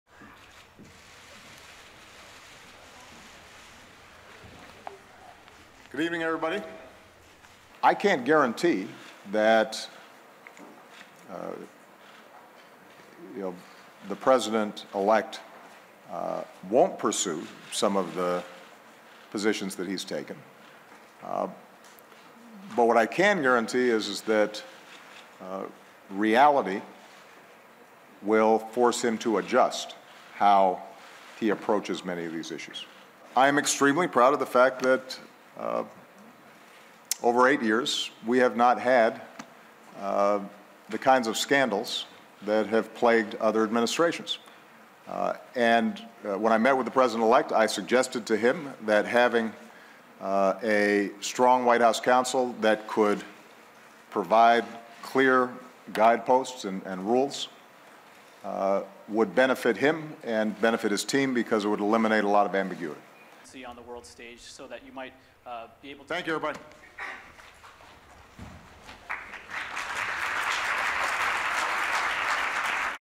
Pour Obama, Trump devra ajuster ses propos à la réalité
Barack Obama a fait ses adieux à la scène internationale dimanche 20 novembre à Lima au sommet de l'Apec, en assurant que son successeur Donald Trump sera forcé d'ajuster ses propos à la réalité. Durée: 01:13